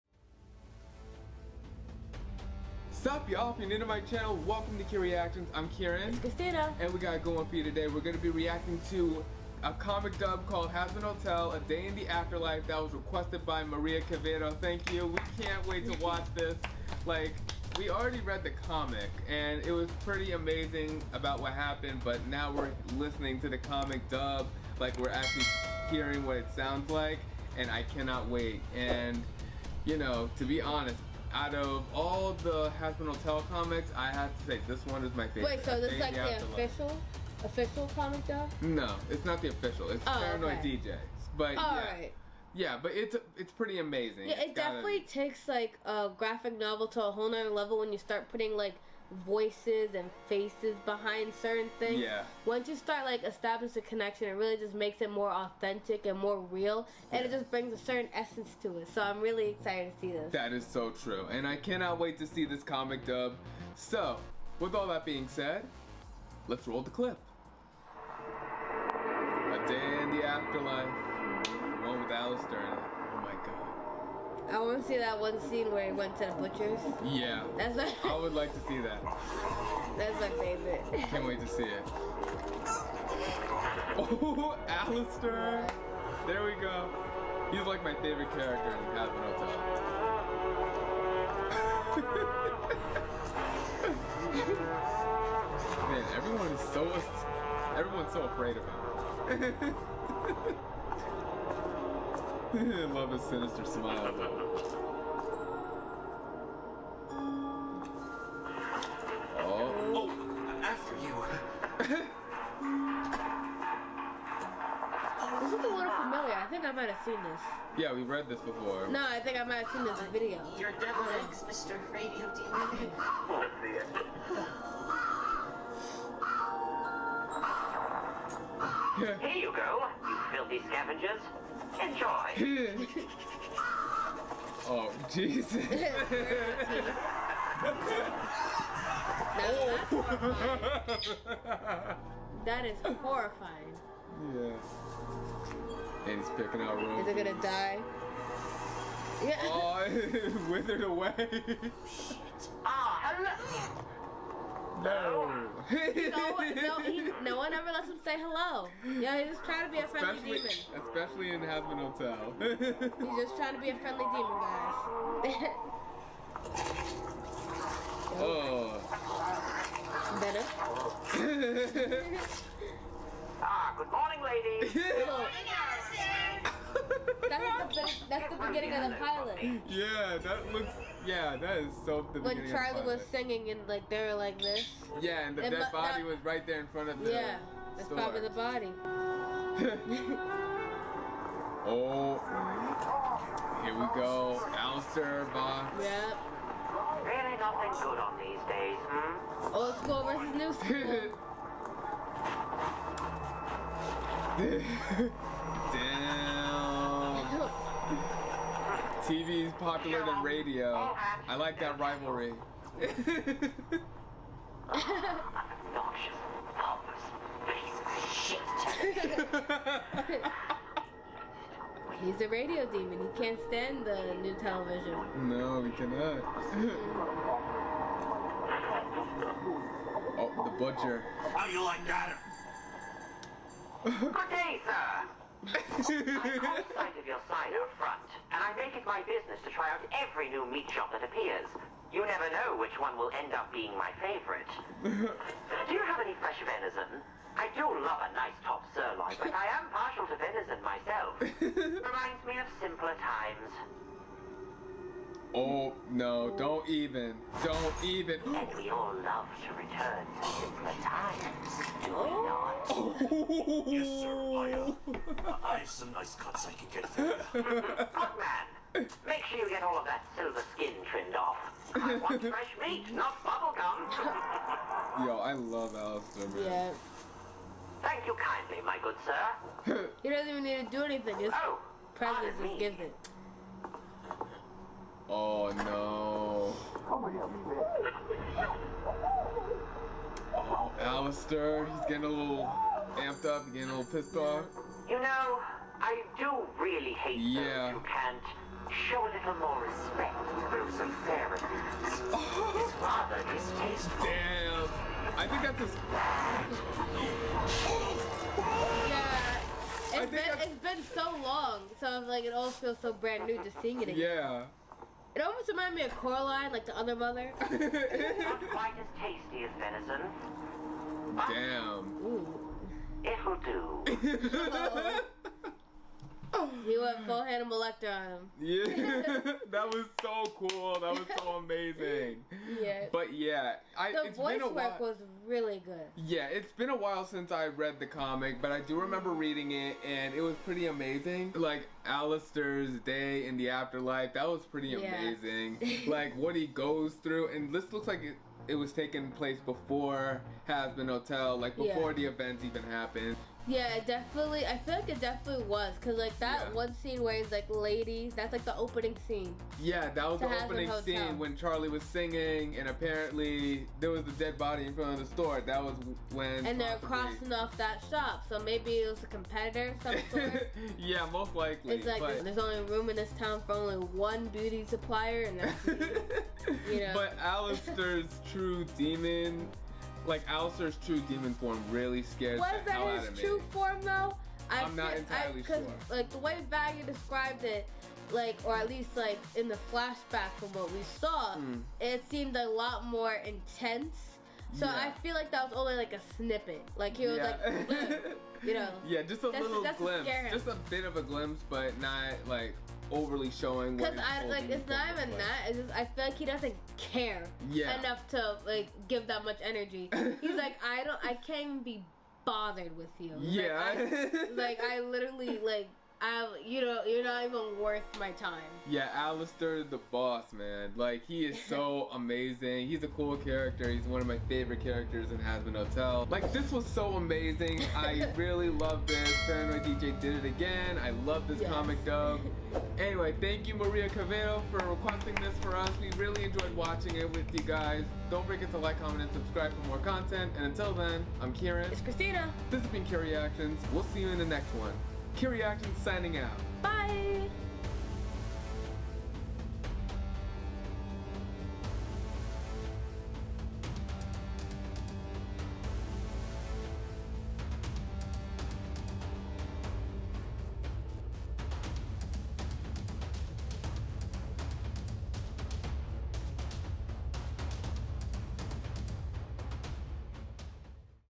HAZBIN HOTEL - "A DAY IN THE AFTERLIFE" Paranoid DJ Comic Dub REACTION/REVIEW!!!
😈 LET"S GO!!! 😈 We're reacting to a Hazbin Hotel Comic Dub! This was requested by Maria Quevedo! Hope y'all enjoy watching our reaction!!!